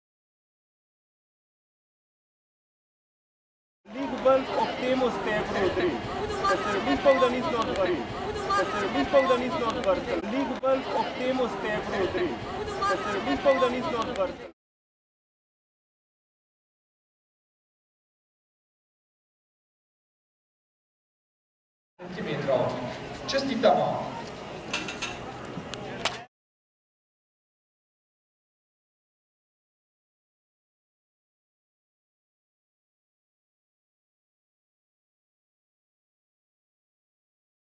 Ana Lilik 130 cm Skok v višino 5. mesto Prvenstvo Ljubljane 2013
Prvenstvo Ljubljane v atletiki-skupina D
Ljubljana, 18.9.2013

Skok v višino starejše učenke:
1 0 BROLIH ANJA VIČ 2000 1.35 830,0
2 0 BAČAK MAŠA O. KOVAČIČ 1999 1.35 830,0
3 0 STARIČ ANA BIČEVJE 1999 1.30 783,0
3 0 BREŠKA ŠILC SAŠKA BARBARA VODMAT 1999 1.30
5 0 LILIK ANA O. KOVAČIČ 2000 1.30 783,0
5 0 ANDLOVIC MAŠA VIČ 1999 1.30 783,0
7 0 SIMONČIČ JANINA T. ČUFARJA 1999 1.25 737,0
7 0 KAMBIČ ANA VRHOVCI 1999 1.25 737,0
9 0 URH TITA TRNOVO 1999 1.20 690,0
9 0 PAVLIČ ARIANA POLJANE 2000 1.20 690,0
11 0 SORČAN ALINA TRNOVO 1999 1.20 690,0
12 0 VINTAR TINA VRHOVCI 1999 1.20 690,0
13 0 GLAVIČ NUŠA T. ČUFARJA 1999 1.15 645,0
13 0 PAVŠIČ TAJA BIČEVJE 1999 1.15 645,0
13 0 KLEMENČIČ DORA M. VRHOVNIK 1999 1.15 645,0
16 0 OLJN ZALA T. ČUFARJA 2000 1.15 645,0
16 0 VELKAVRH NINA VIČ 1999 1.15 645,0
18 0 ZMIJANJAC MAJA M. VRHOVNIK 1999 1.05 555,0
0 STEBERNAK TEA M. VRHOVNIK 1999 NN 0
0 LOZEJ TISA POLJANE 2000 NN 0
0 KOSI KAJA O. KOVAČIČ 1999 NN 0
0 ORNIK JULIJA TRNOVO 2000 NN 0

HIGH JUMP - HOCHSPRUNG - Прыжок в высоту - SKOK U VIS - 跳高 - SALTO IN ALTO - SALTO DE ALTURA - SAUT EN HAUTEUR - ऊंची कूद -